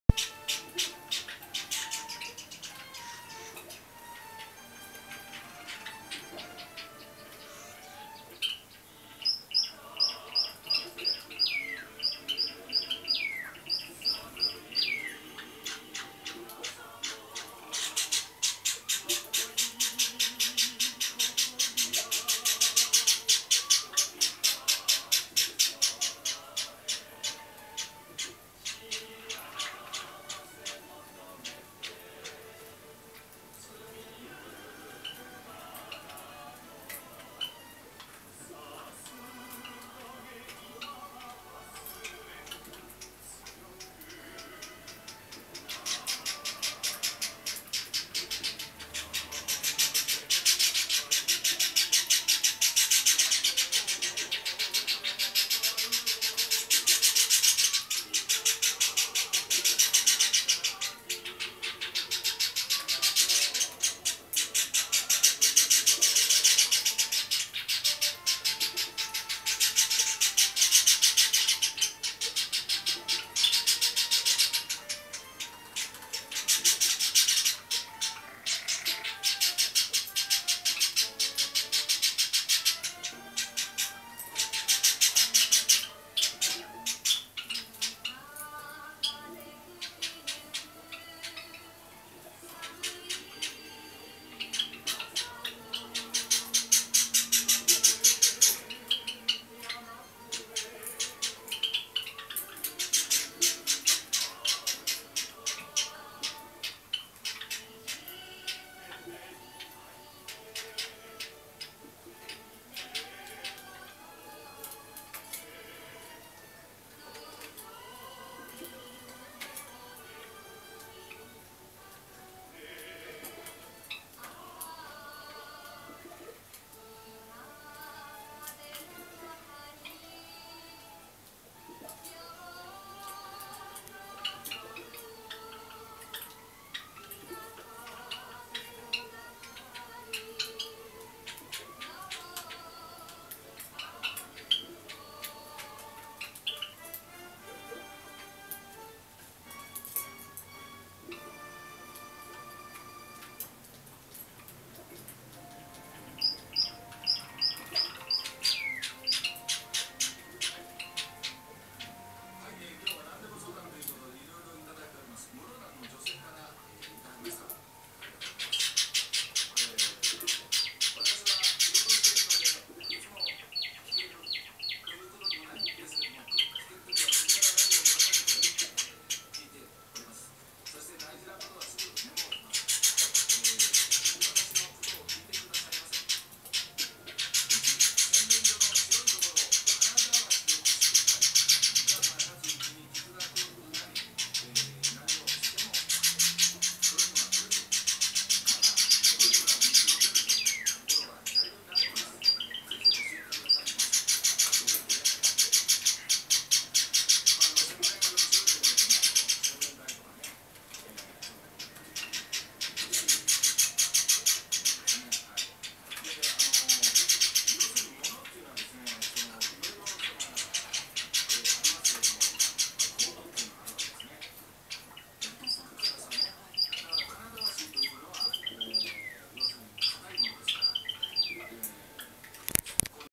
文鳥雛生後32日目と30日目の鳴き声
朝10時25分、今日初めてのゲボゲボ・・・・よかった～
2012年1月